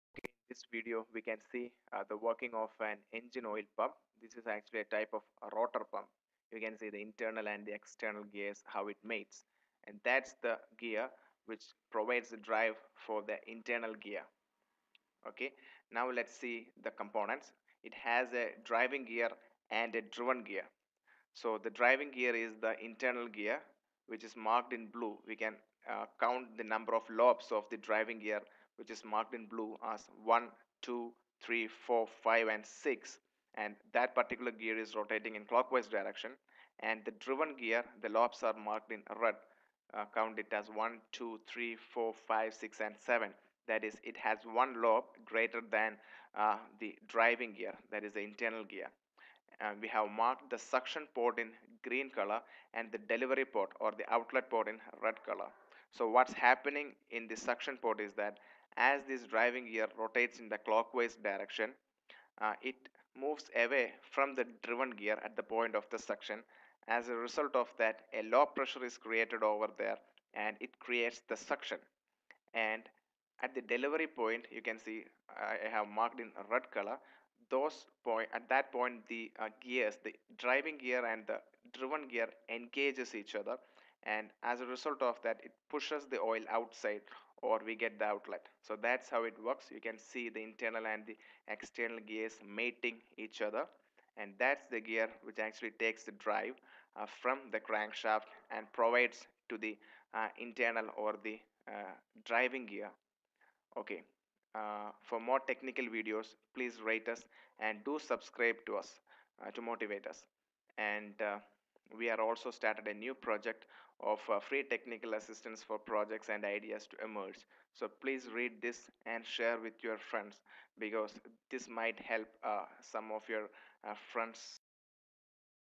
How an engine oil pump works. ✔
This video explained how an engine oil pump works.
This video was taken while assembling a diesel engine, engine oil pump is a rotor type pump.
Similar and related topics of this video.